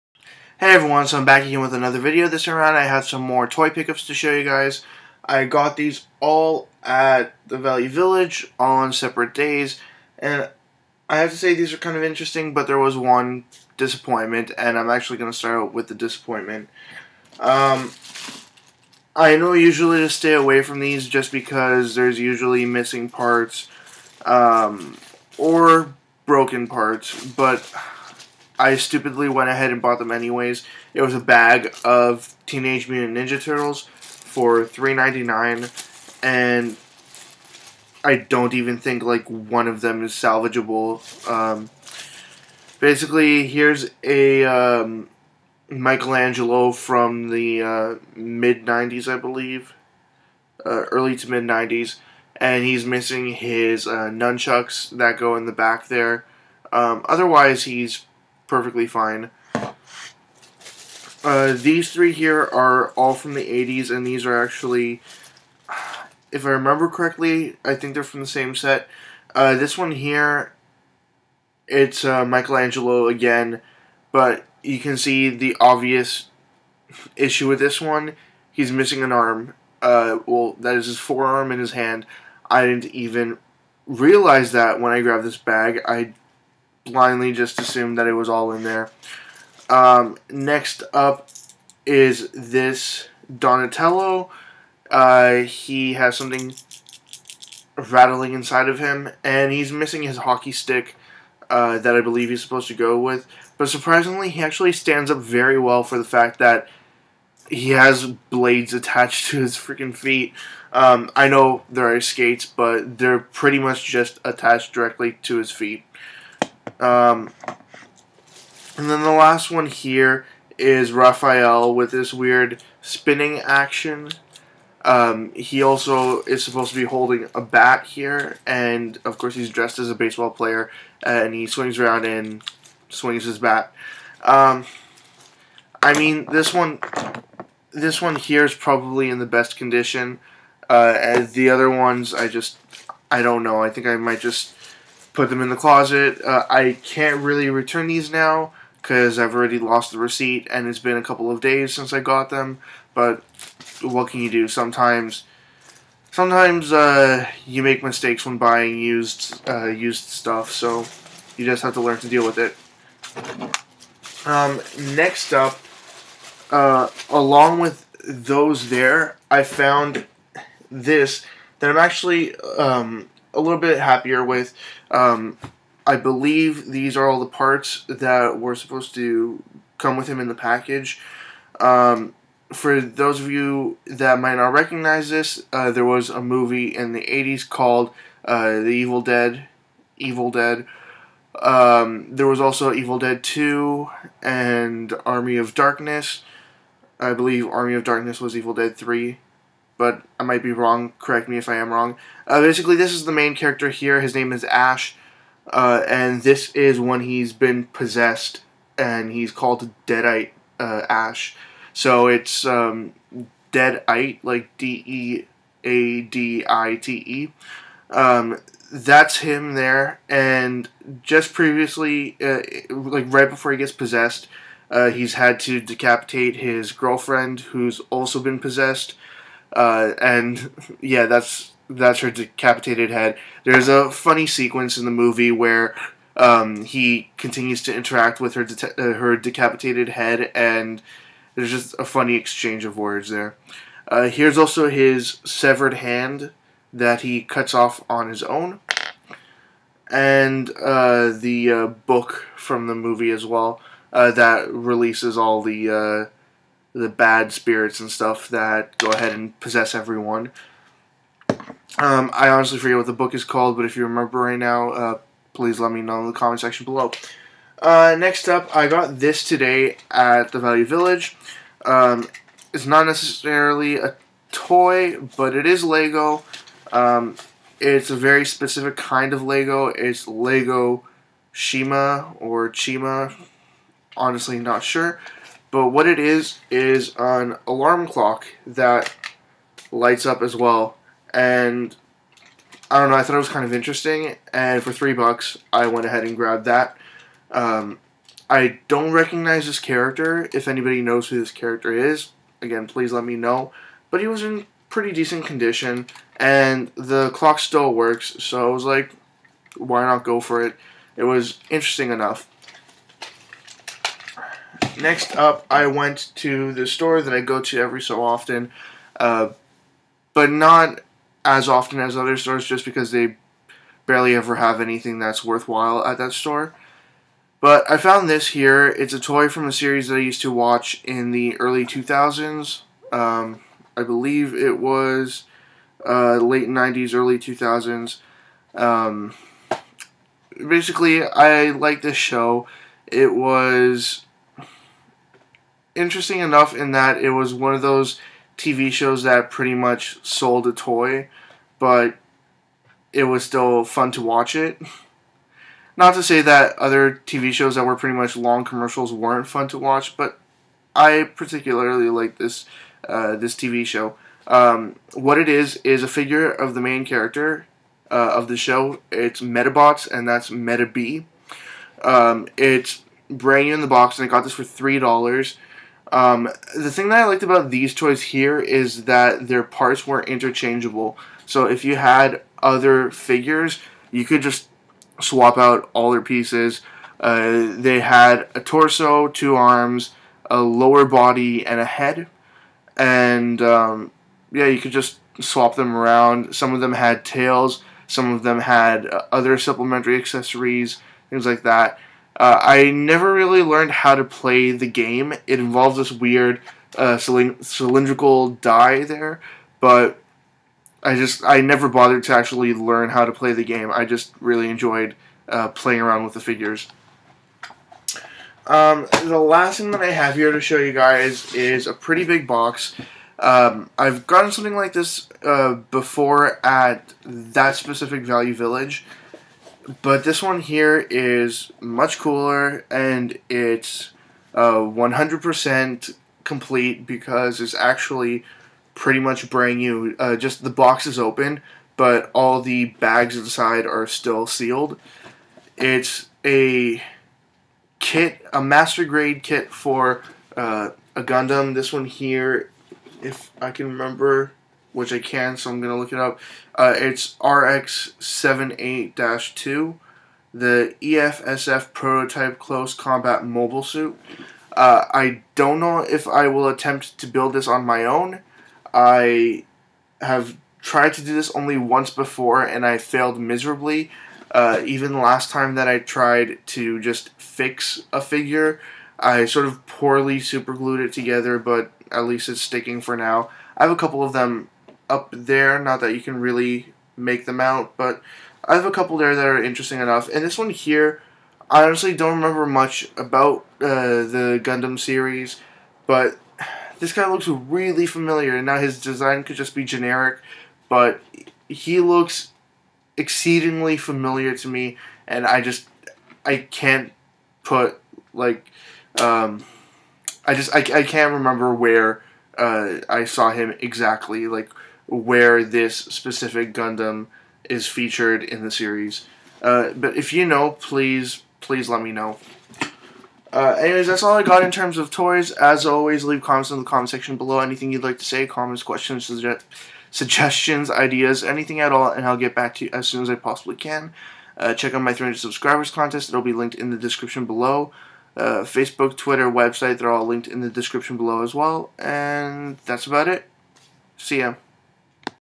Toy Pickups #8: TMNT Fail!
Got some more toys, but I did make a rookie mistake. Remember to always check for condition!